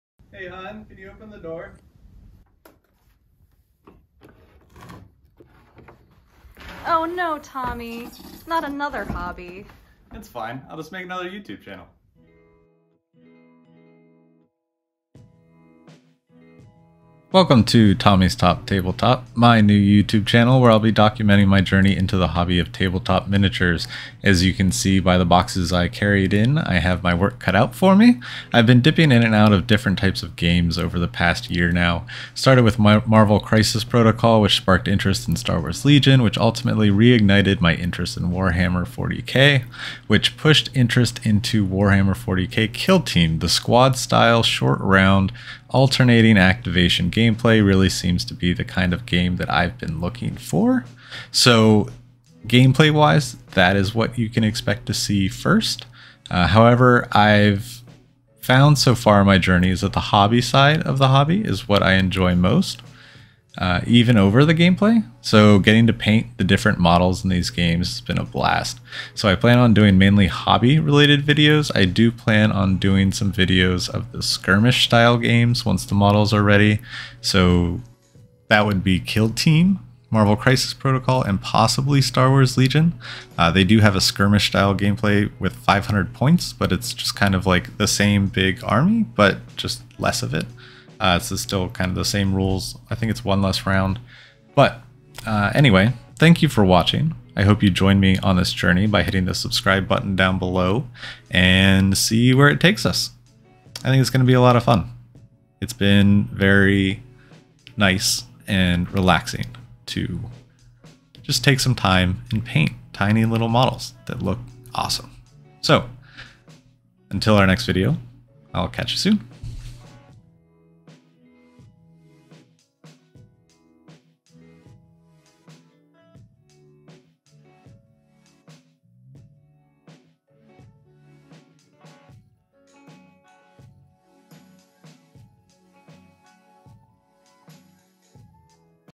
Tommy's Top Table Top Channel Trailer
Hey it's Tommy from Tommy's Top Picks. This is my new YouTube channel where I'll be painting and talking about my journey into the hobby of table top miniatures!